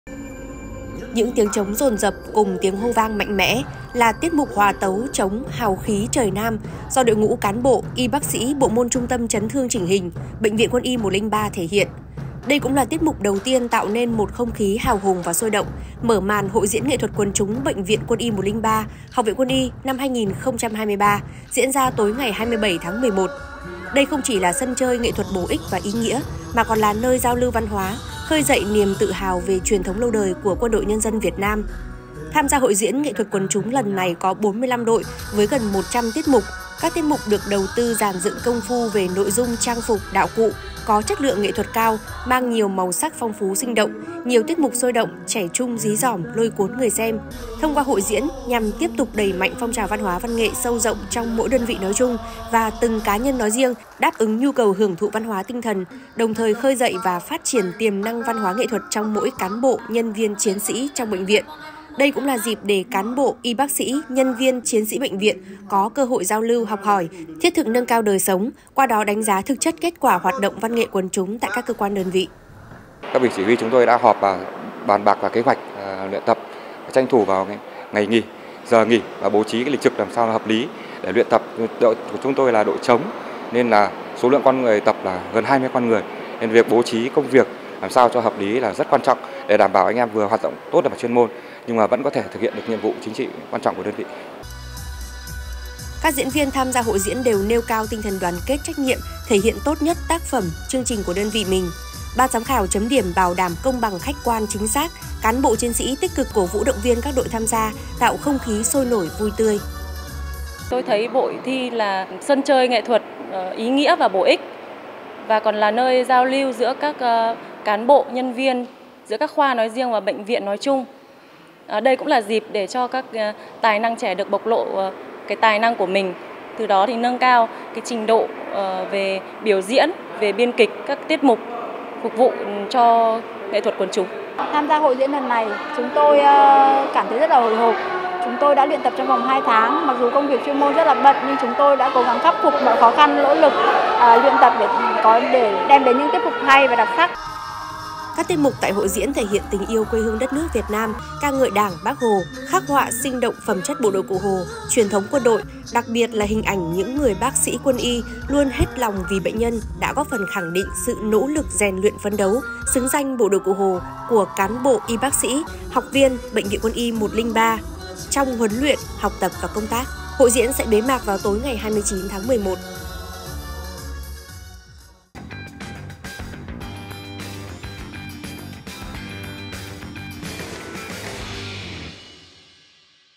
Nhiều tiết mục đặc sắc tại Hội diễn nghệ thuật quần chúng Bệnh viện Quân y 103 - Báo QĐND
Báo QĐND - Tối 27-11, tại Hà Nội, Bệnh viện Quân y 103 (Học viện Quân y) tổ chức Hội diễn nghệ thuật quần chúng năm 2023.